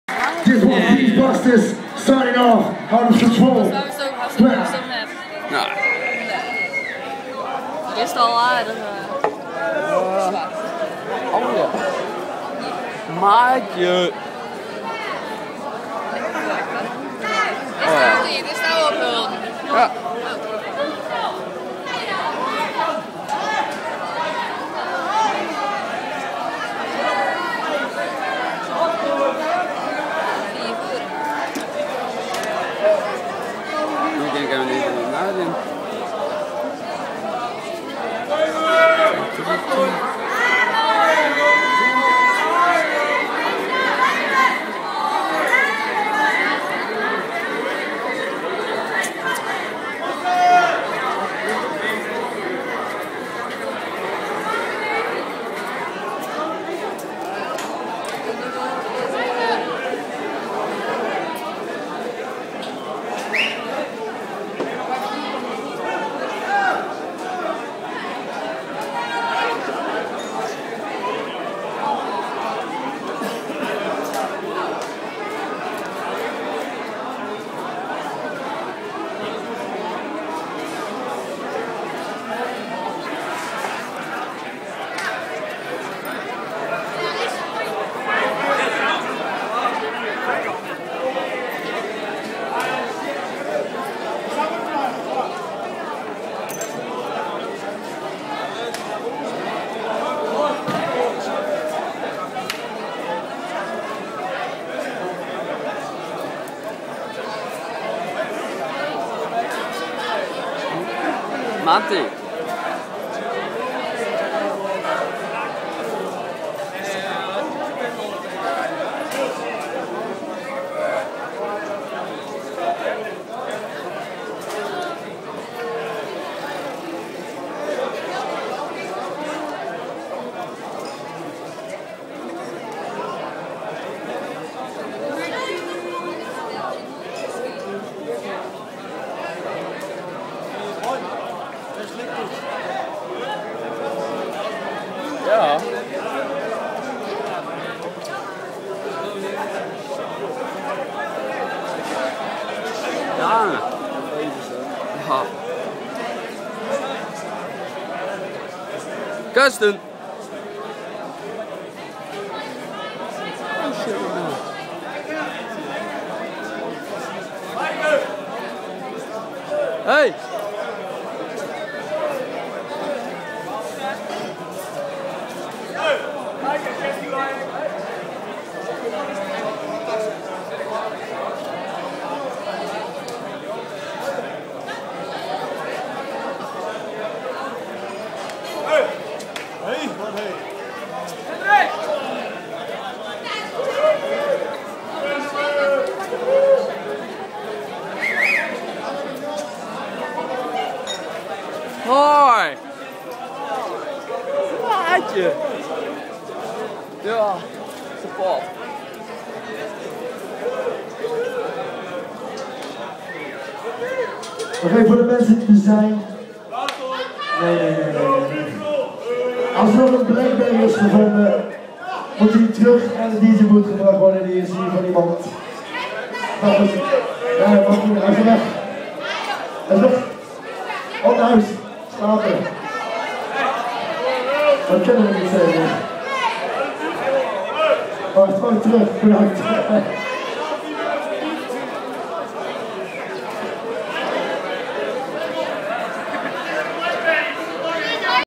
op het einde in sans souci 15-10-2011
ff een doeloos filmpje van toen de muziek ophield enz.. ff de frisians 4 hardstyle members filmeh:P gaat echt ff nergens over whahaha. kiek maar es ff..